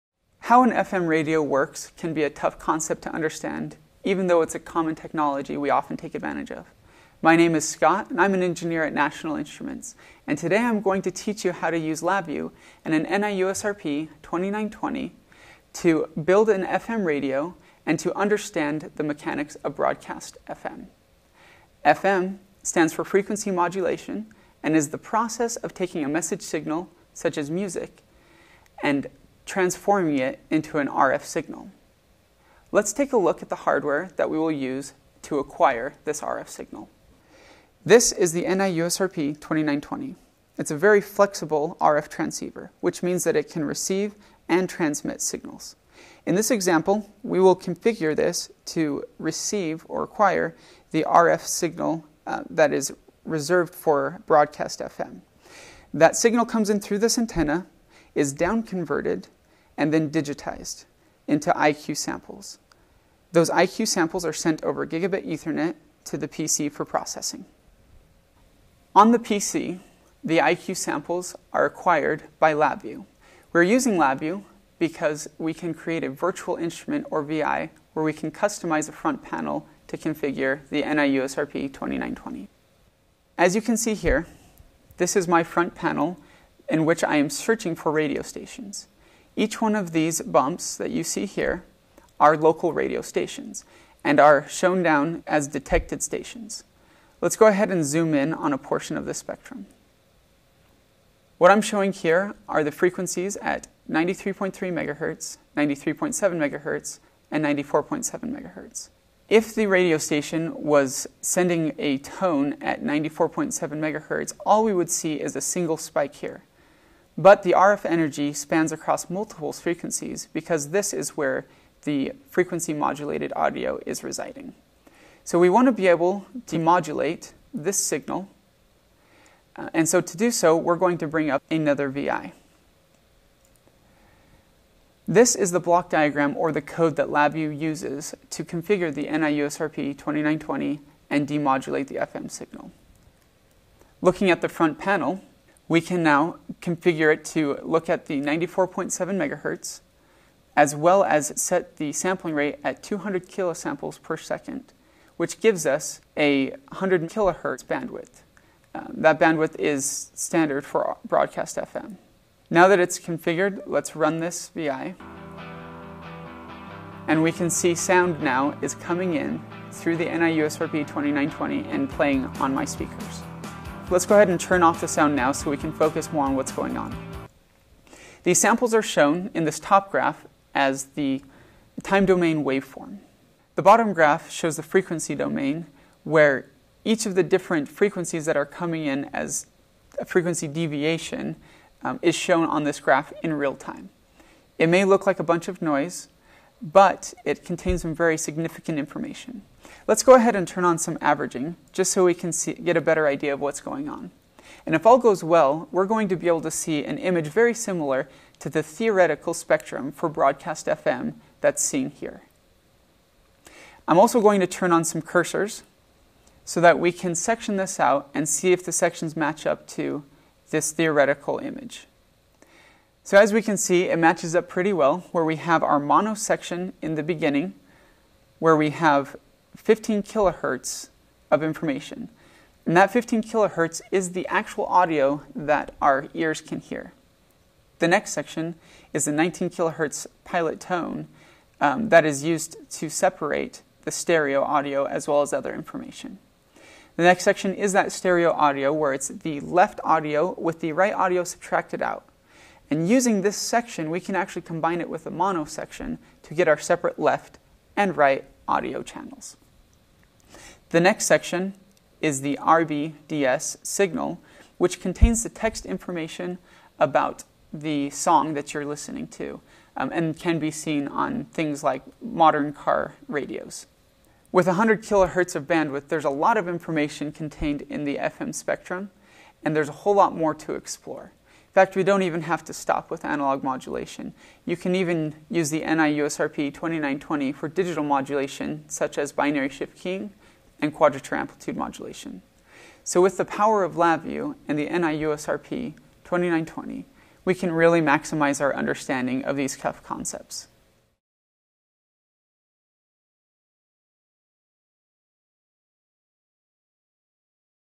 Tough Concepts: Understanding How Broadcast Frequency Modulation Works
How FM radio works can be a tough concept to understand, even though it's a common technology we often take advantage of. In this video, you can learn how to use LabVIEW and the NI USRP-2920 to build an FM radio and gain a better understanding of the mechanics of broadcast FM.